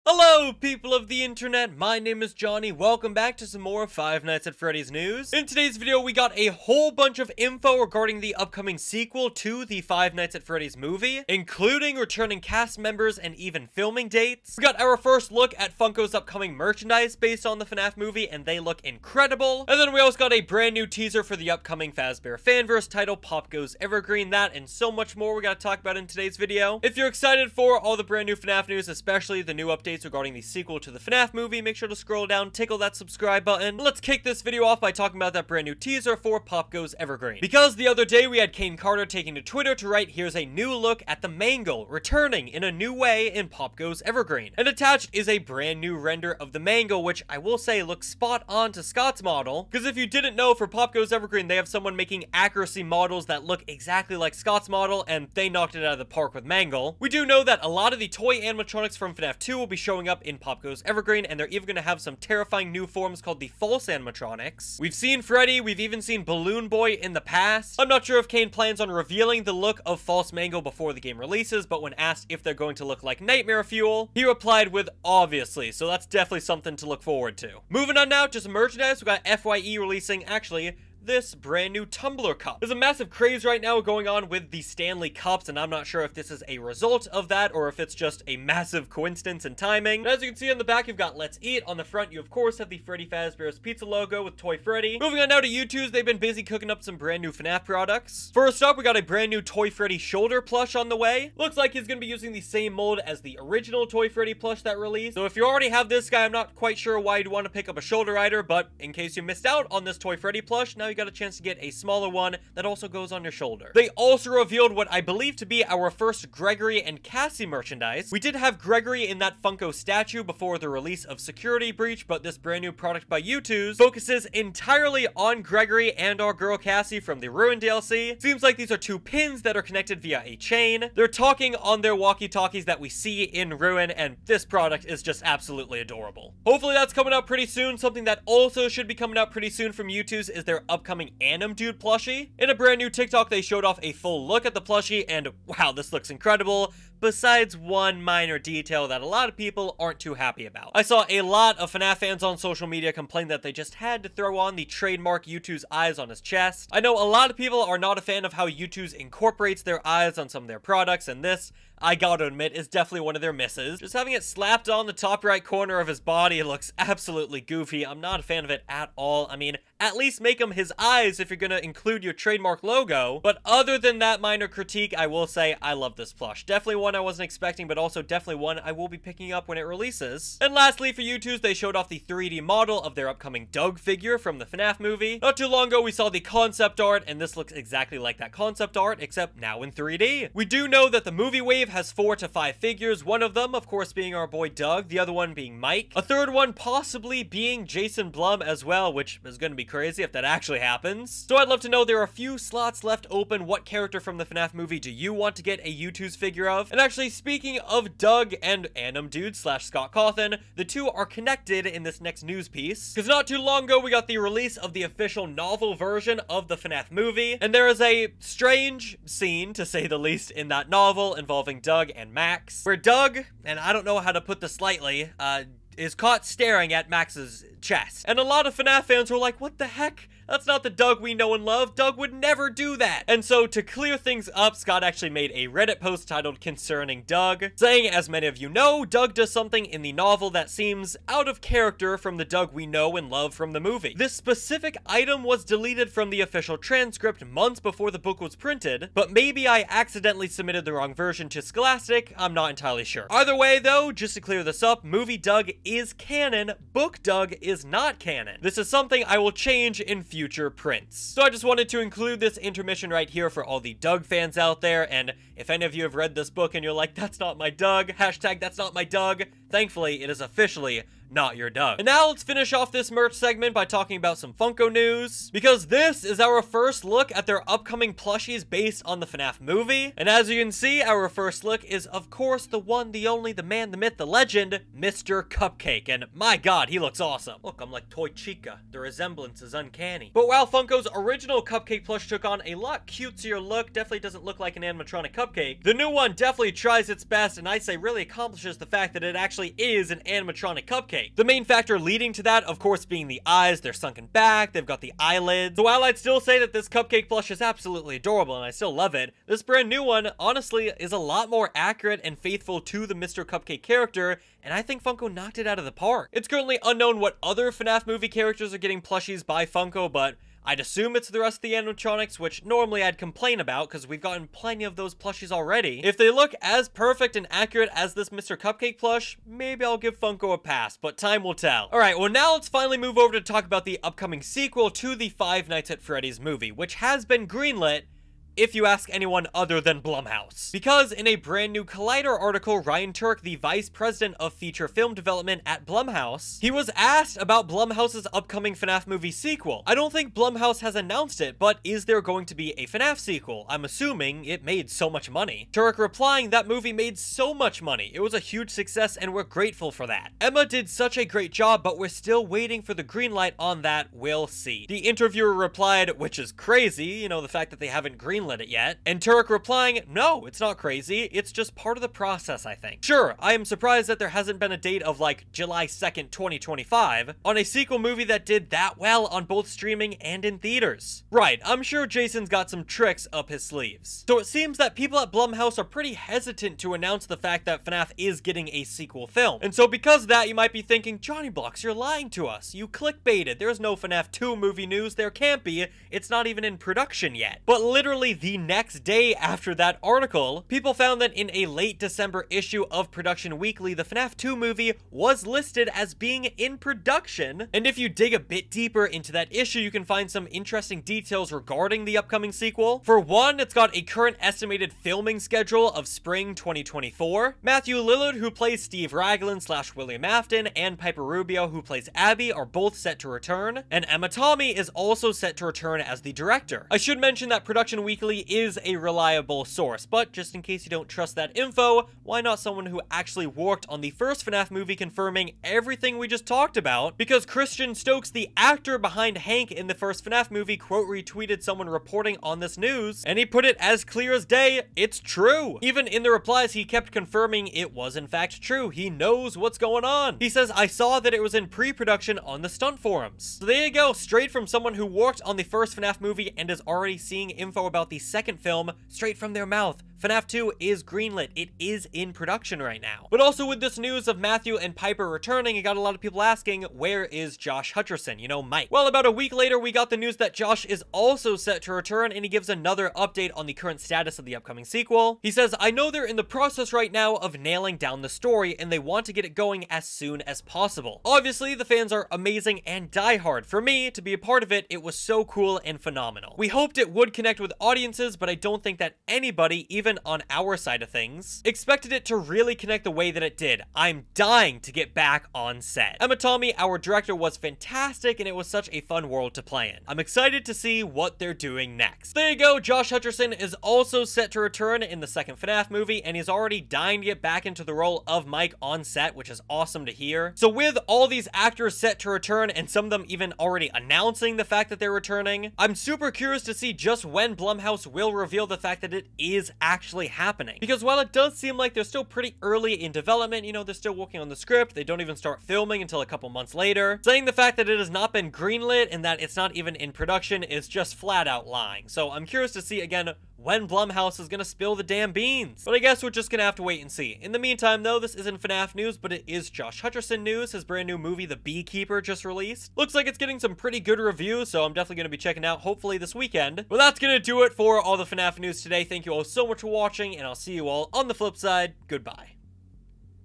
NEW FNAF MOVIE SEQUEL DETAILS! Cast Revealed, Leaked Merch, Filming Dates, & MORE! - FNaF News
Five Nights at Freddy's (FNAF) News! Fazbear Fanverse POPGOES Evergreen, FYE FNaF, YouTooz FNaF Plush, Leaked Funko FNaF Movie Merch, FNaF 2 Movie Details, & MORE!

0:00 POPGOES Evergreen Mangle Teaser
1:17 FYE FNaF Tumbler Cup
1:35 Youtooz Movie Figures, Animdude Plush, & More
3:26 Scott Cawthon Addresses FNaF Movie Novel Drama
4:31 Funko FNaF Movie Plushies Leaked
5:31 FNaF Movie Sequel New Details